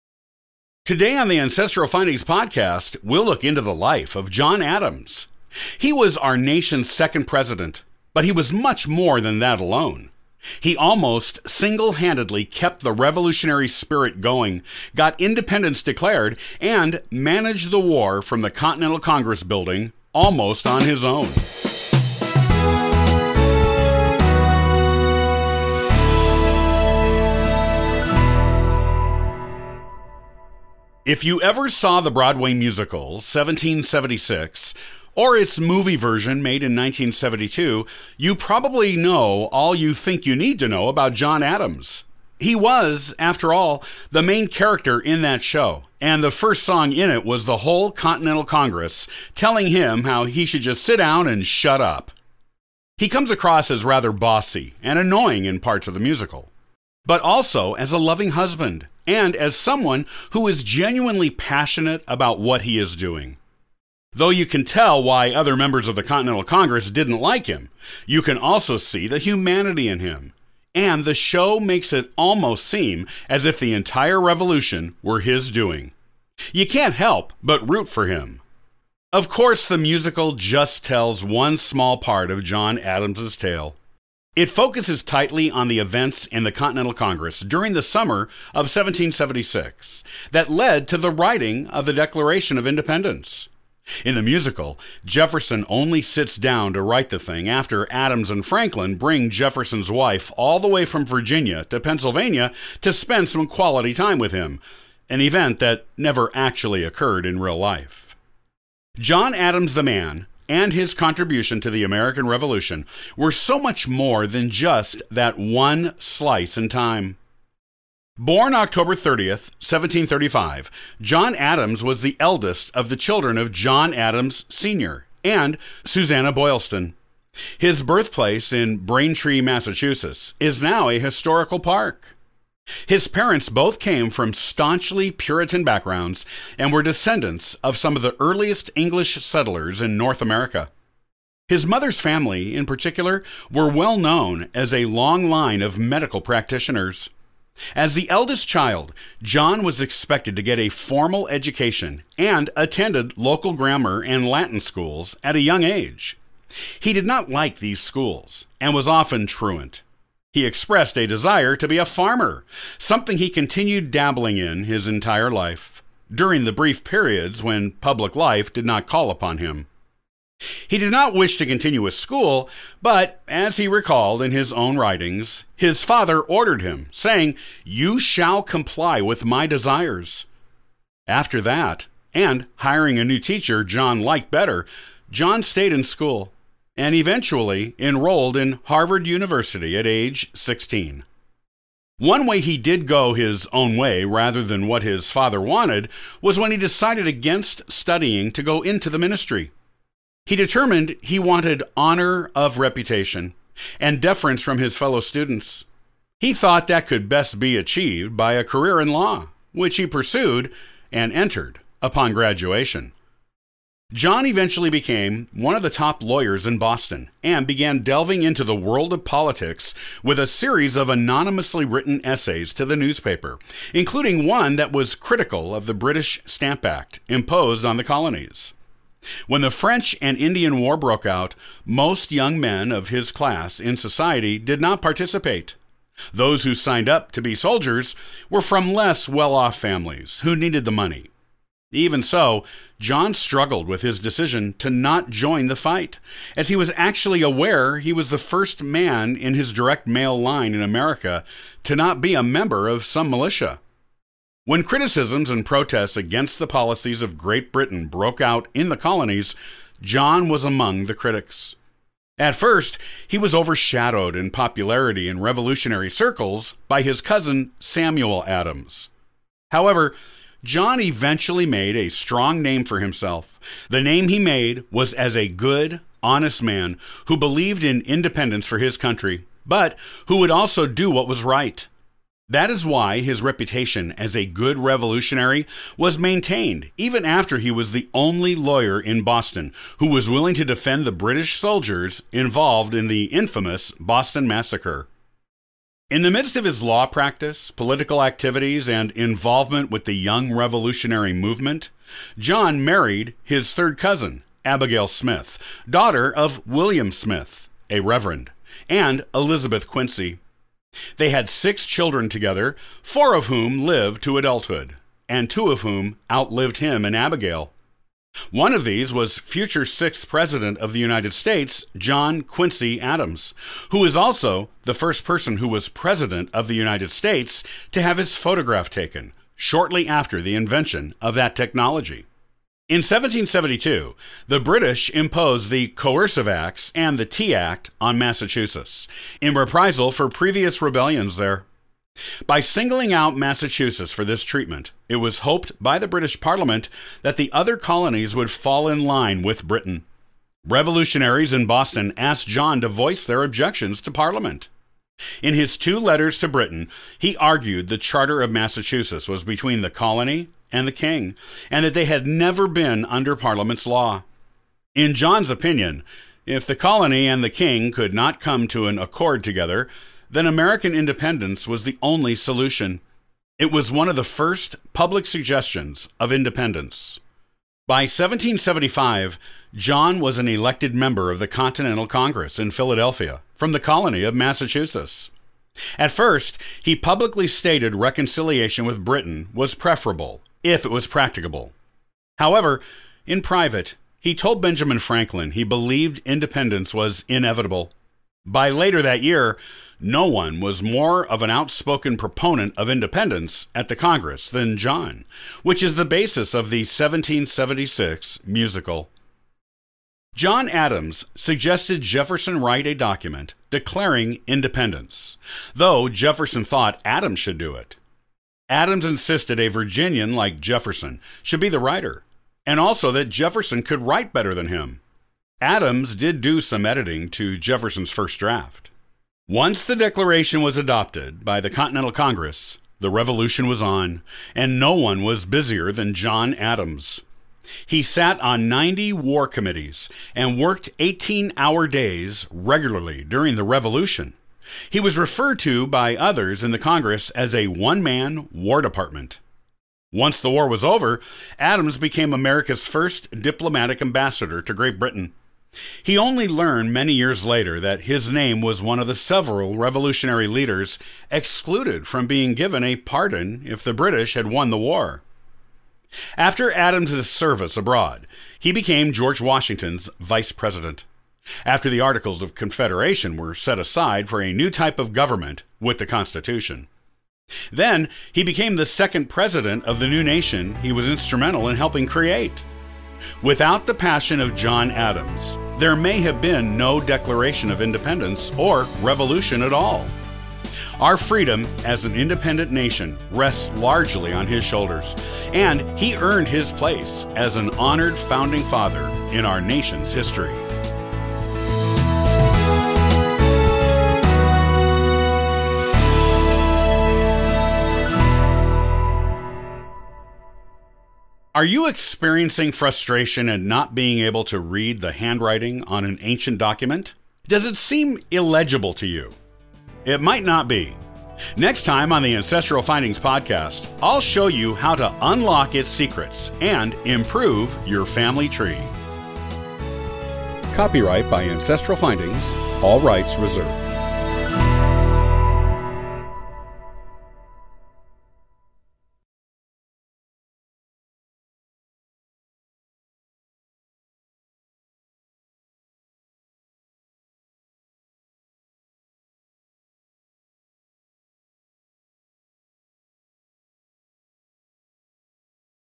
John Adams: Who's Who in the American Revolution | Ancestral Findings Podcast
AF-187: John Adams: Who's Who in the American Revolution | Ancestral Findings Podcast

Today on the Ancestral Findings Podcast we’ll look at the life of John Adams. He was our nation’s second president, but he was much more than that alone. He almost single-handedly kept the revolutionary spirit going, got independence declared, and managed the war from the Continental Congress building almost on John Adams own.